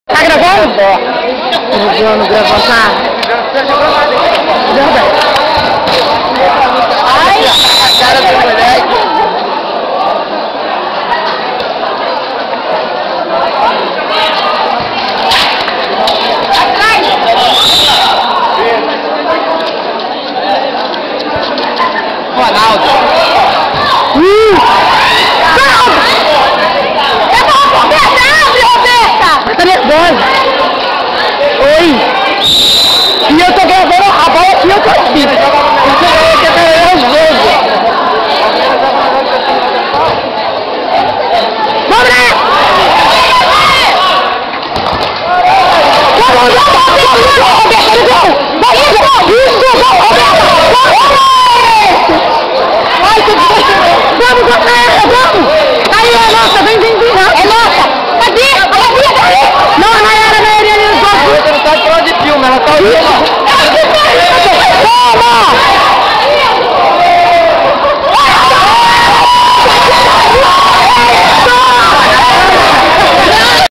IDH - Futebol (3)
Campeonato de Futebol Feminino -  2002 X 3002 - 6 a 0 2002